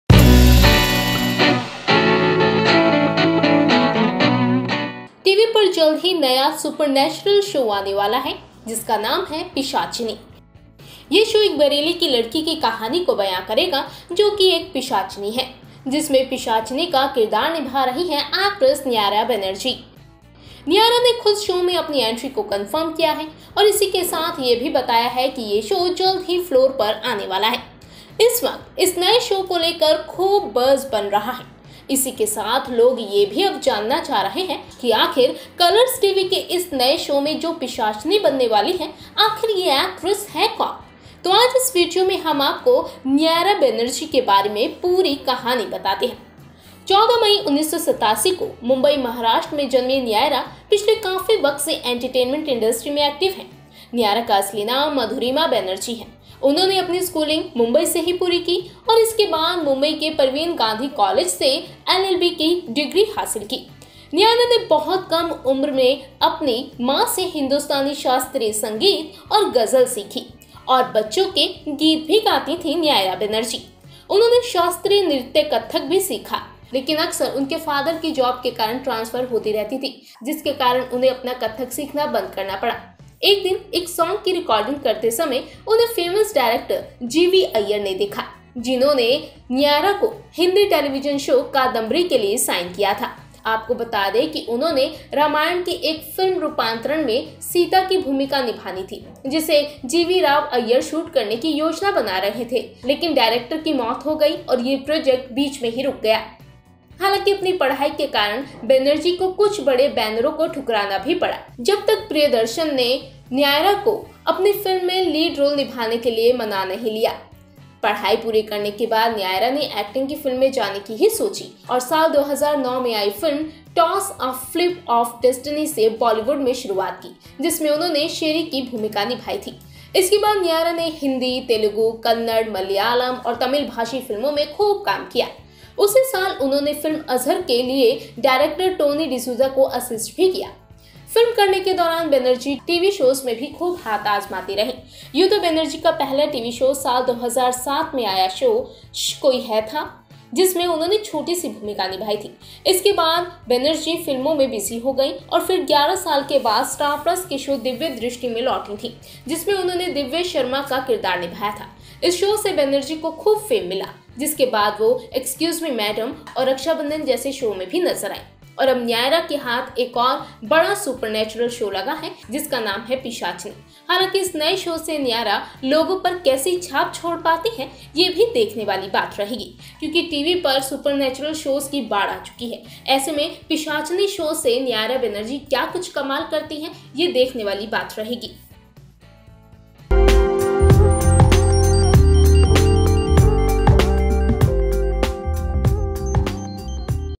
Who is Nyra Banerjee, who will be seen as a PISHACHINI in the new show of Colors!
Check Out About All Thing of New Actress Nyra Banerjee, Who is Going to Play The Role of PISHACHINI in New Show!